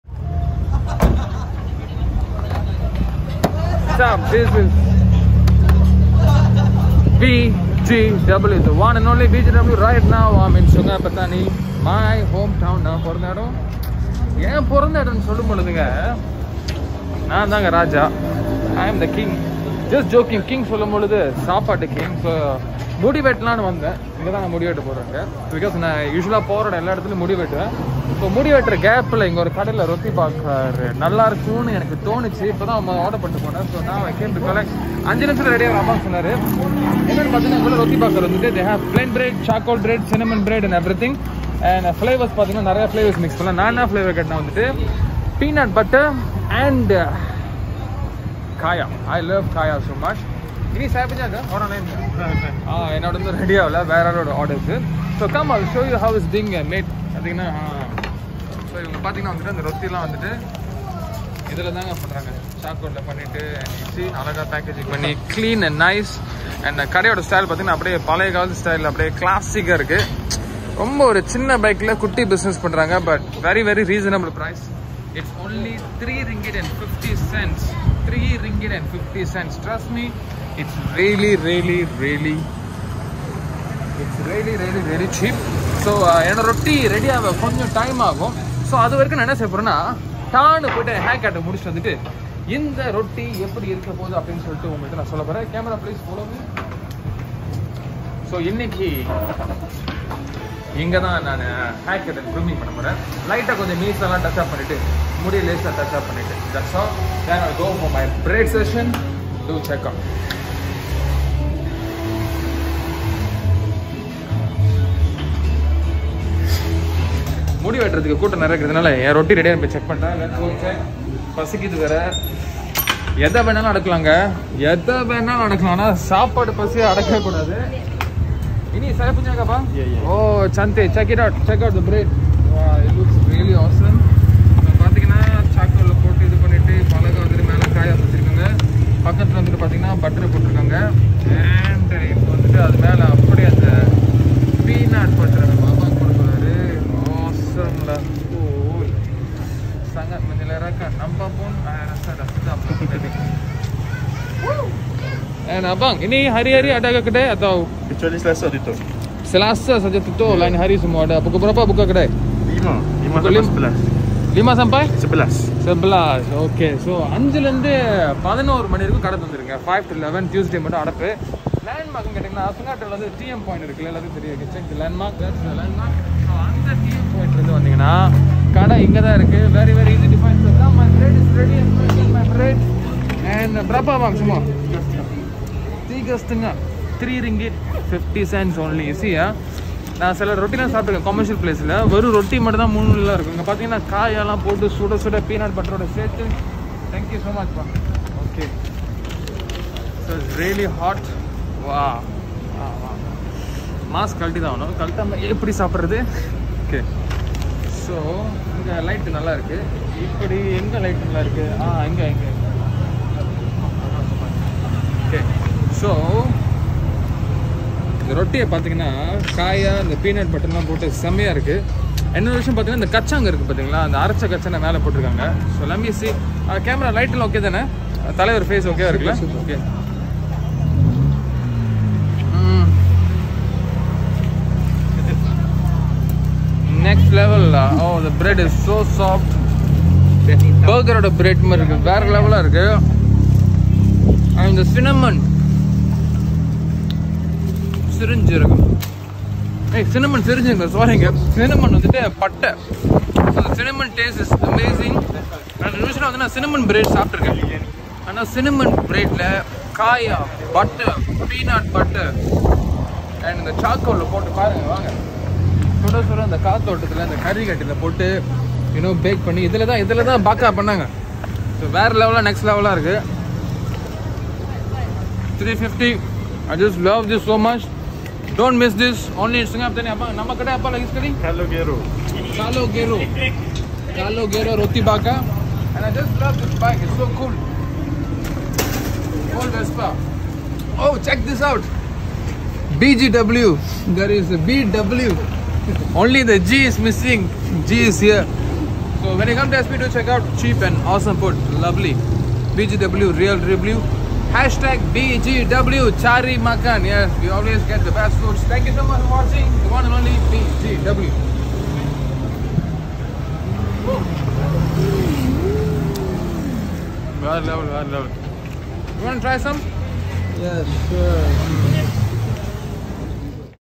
BEST Roti Bakar 😎 BGW Cari Makan Malaysia #1 @ Sungai Petani Kedah BGW 🅱️
BGW picks the best local food in Malaysia, a place known as food paradise! In this episode, BGW randomly ordered a Tepi Jalan Roti Bakar and guess what, BGW Impressed with the taste!
This is a real review of BGW! One of a kind BGW Food Review! 🤙🏻